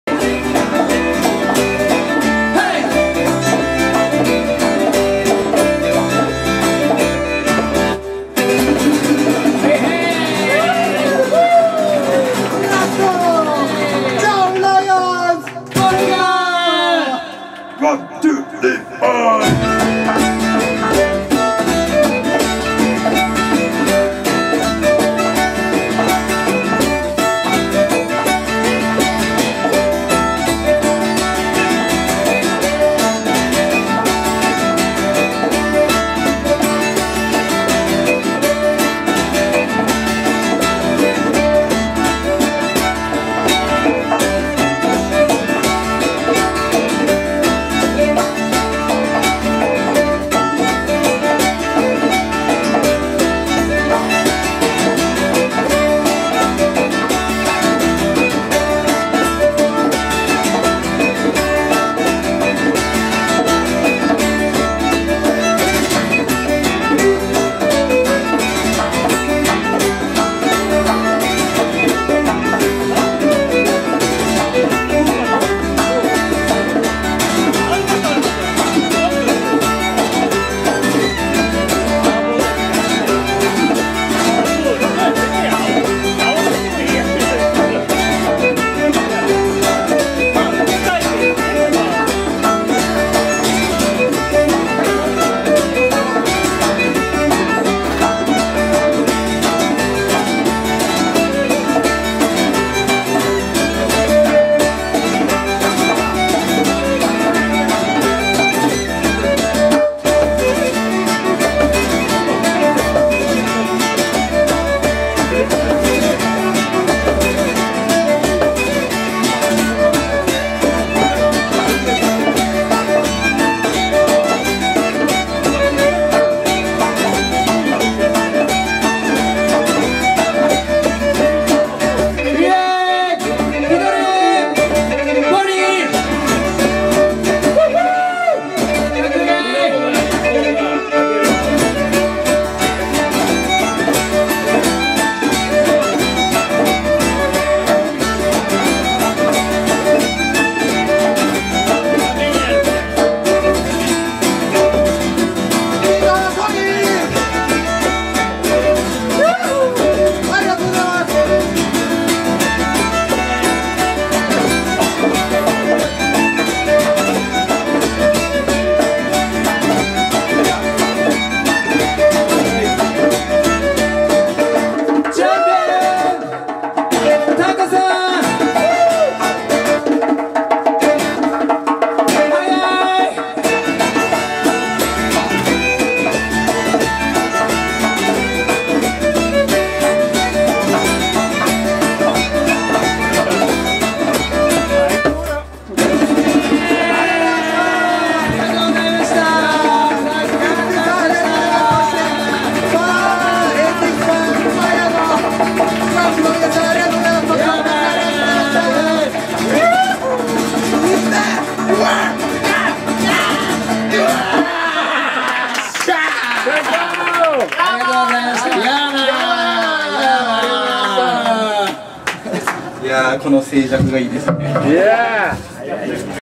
2016.9.3 ザ・ガーデナーズ Live in musicbar MA-YA 5
２０１６年９月３日(土)。
親不孝通りのミュージックバー、マーヤにて。
タカさん入っての三人編成。